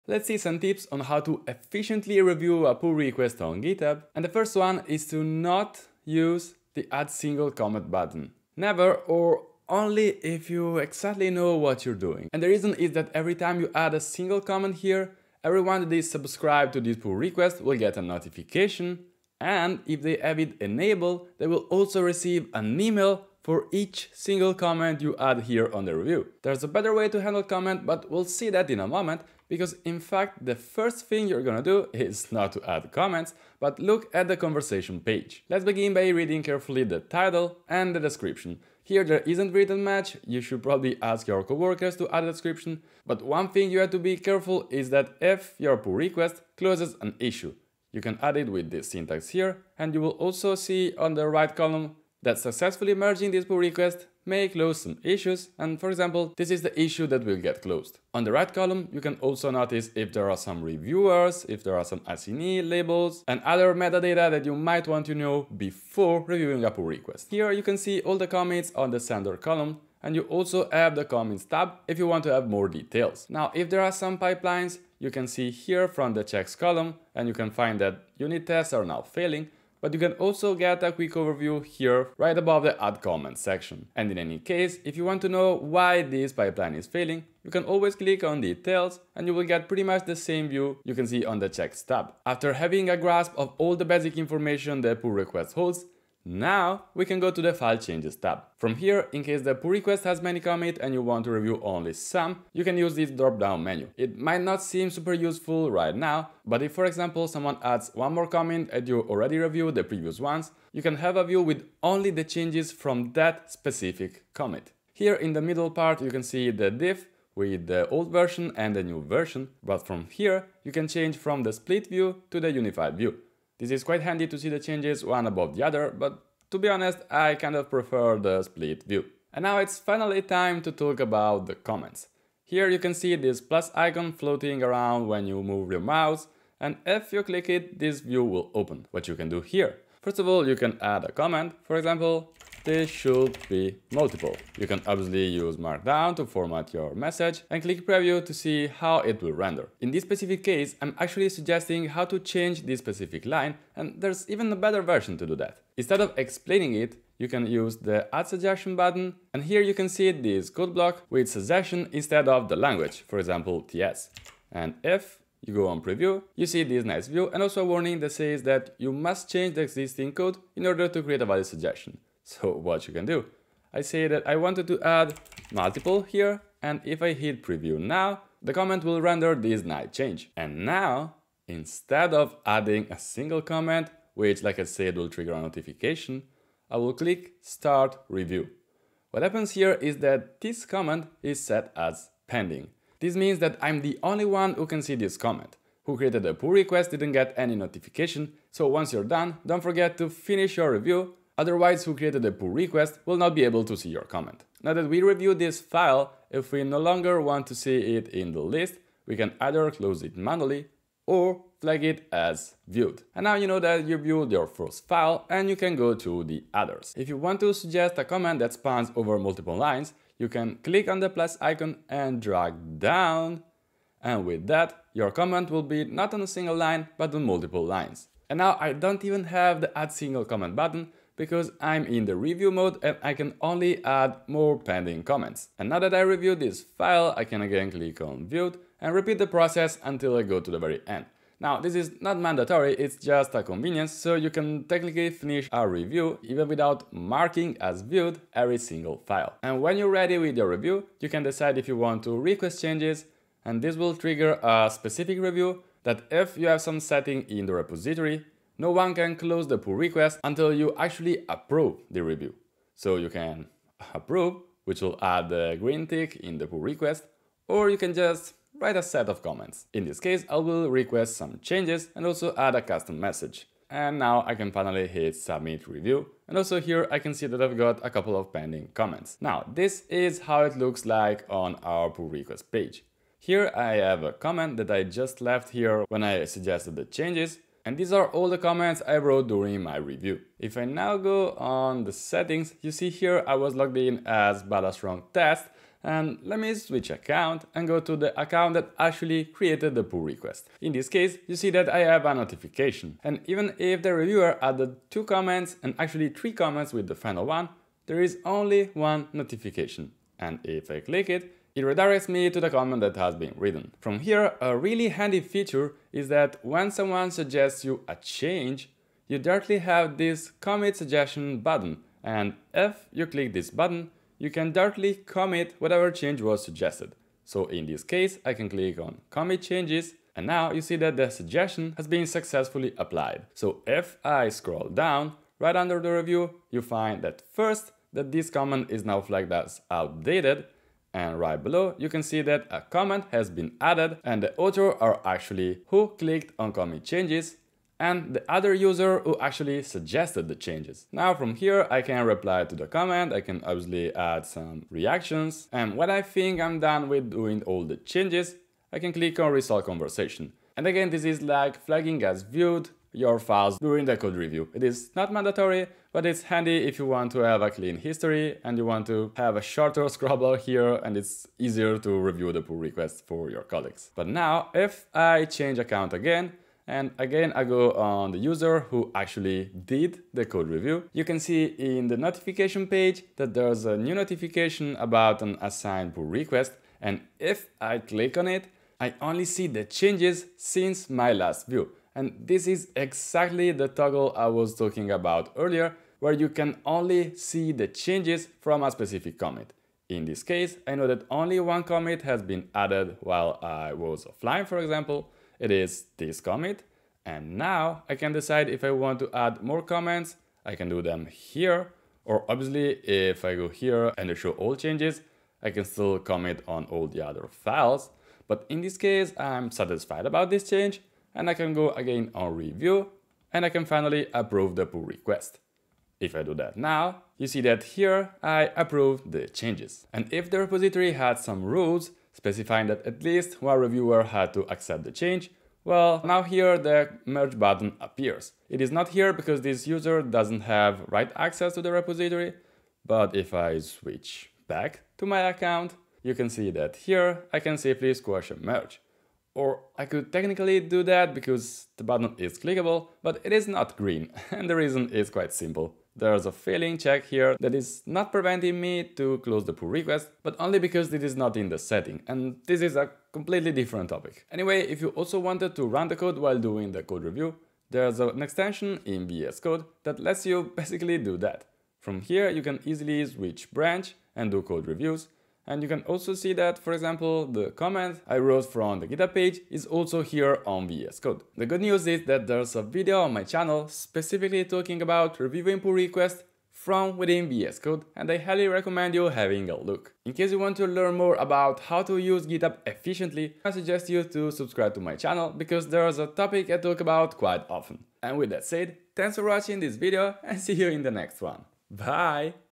How to Review a Pull Request on GitHub
Reviewing a PR on GitHub is an activity we do daily, hence having good knowledge of the tool increases our efficiency and ensures smooth collaboration with our colleagues.

In this beginner's guide, we'll explore the core features GitHub offers and some practical tips to enjoy the process.

Conducting a code review is actually a really tough task, it not only requires programming knowledge but also a strong cultural maturity, for instance when it comes to criticizing the code and not the author.

Empathy is among the hardest skills one has to master, but knowing the tool is one of the first steps in making the process easier.

TIMESTAMPS
00:00 Don’t use "add single comment"
00:35 Pull Request Data
01:07 Commits & Pipelines
01:32 File changed tab
02:09 Adding comments
03:00 Start review
03:36 Multi-lines comment
03:55 Finish the review (Approve, Request changes or Comment)
04:50 Reply to a review & accept suggestions
06:13 Review additional changes
07:09 Finalizing the review
07:44 Review from Visual Studio Code